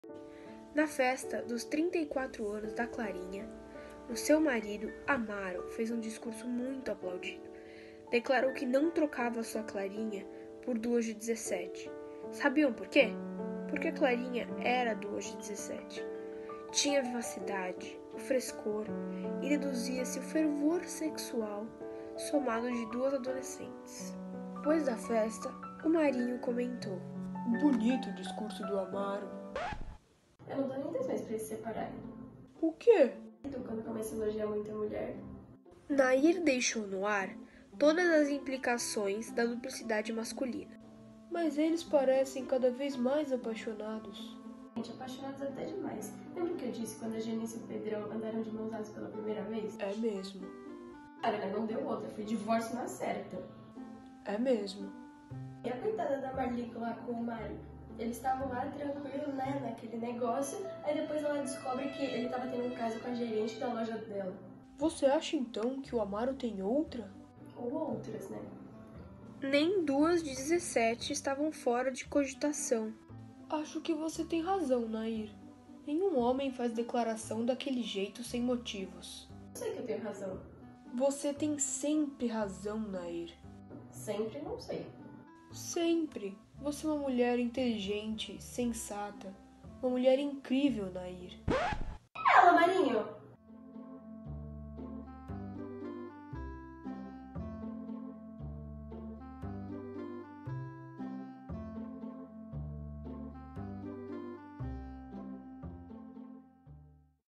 Mini Curta Beijinho beijinho G3
Micro curta teste sobre texto de Luís Fernando Veríssimo para as aulas de Transposição de Linguagem do Colégio Santo Ivo. Grupo formado por
Tiago
Rafaella Habeyche
Isadora Trevisan
Julia Khoury